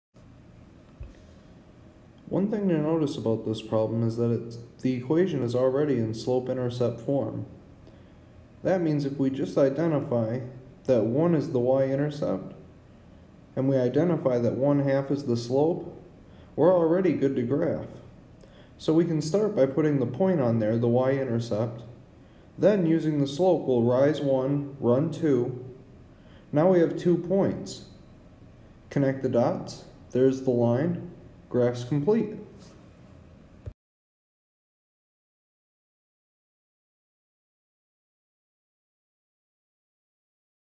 Line: Graph the line y = 1/2 x + 1
Tony from SVSU Micro Math helps you graph a line using slope-intercept form.

Problem: Graph the line y = 1/2 x + 1